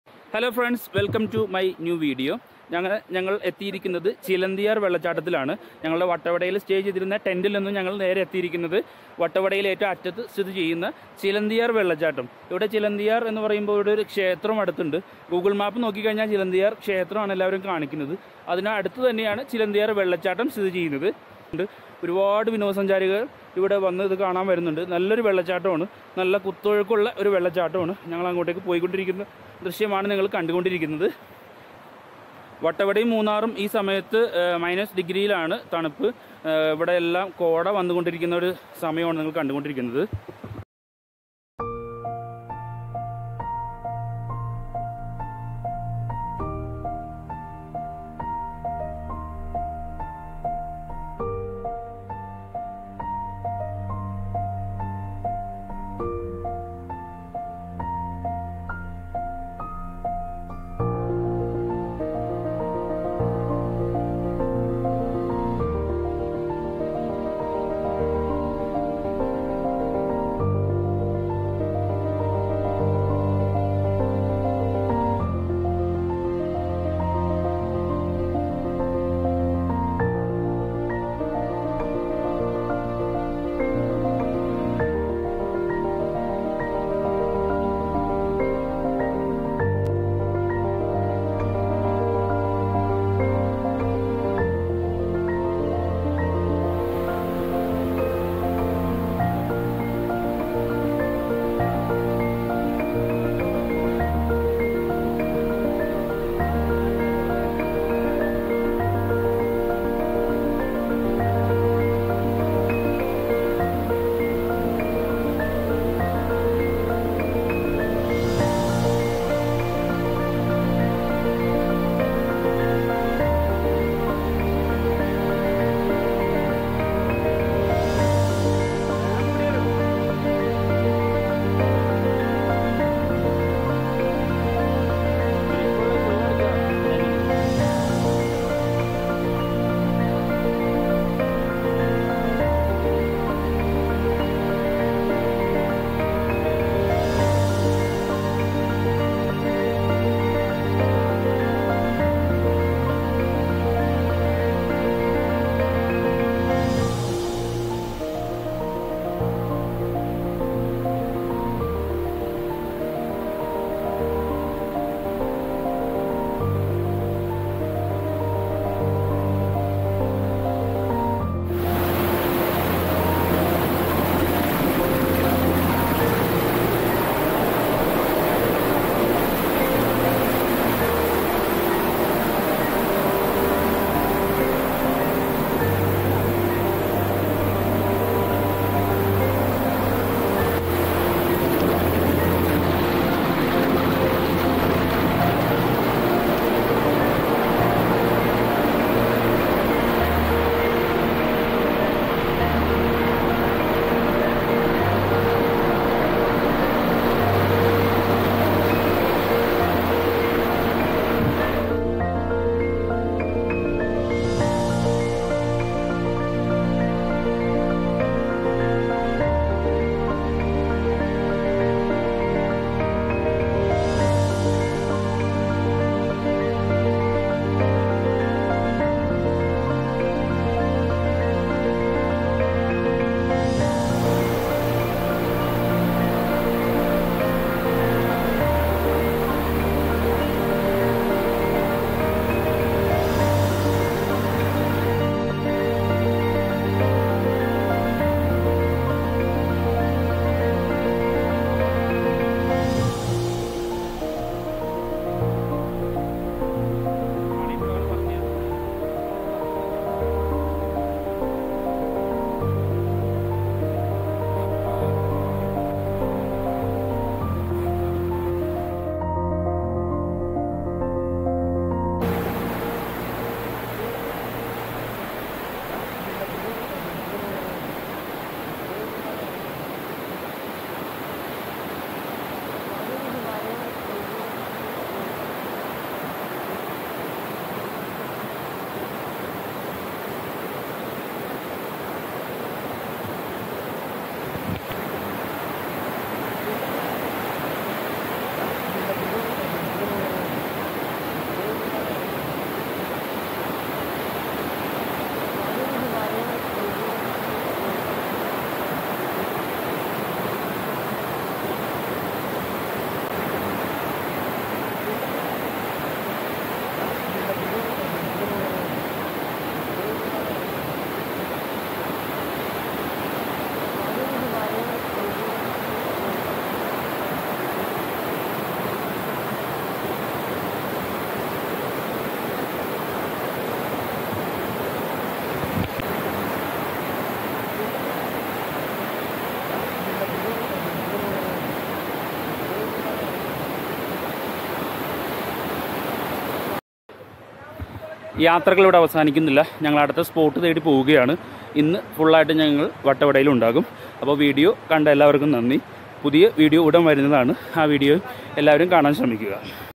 Chilanthiyar Water Falls | ചിലന്തിയാര്‍ വെള്ളച്ചാട്ടം | Vattavada| Shejeeb Edathala|3 days Exploring
ചിലന്തിയാര്‍ വെള്ളച്ചാട്ടം
 വട്ടവടയില്‍ നിന്ന് കോവിലൂര്‍ വഴി ഏഴു കിലോമീറ്റര്‍ സഞ്ചരിച്ചാല്‍ ചിലന്തിയാര്‍ വെള്ളച്ചാട്ടത്തിലെത്താം. മനുഷ്യരുടെ ഇടപെടല്‍ അധികം എത്തിപ്പെടാത്ത പ്രദേശം. പക്ഷികളുടെ കലപില ശബ്ദങ്ങള്‍ കേട്ടുണരുന്ന, ഉച്ചയ്ക്കും കോടമഞ്ഞിനാല്‍ മൂടിയ താഴ്‌വാരം. വെള്ളച്ചാട്ടത്തിന് അടുത്തുവരെ കോണ്‍ഗ്രീറ്റ് റോഡുണ്ട്. ഈ റോഡിലൂടെ വാഹനവും സഞ്ചാരയോഗ്യമാണ്. കാഴ്ച മറയ്ക്കുന്ന മൂടല്‍ മഞ്ഞും നീരൊഴുക്കിന്റെ കളകളാ ശബ്ദവും ഇടതൂര്‍ന്ന മരങ്ങളും സ്വര്‍ഗീയ അനുഭൂതിയൊരുക്കുന്നു. പാറക്കെട്ടുകള്‍ക്...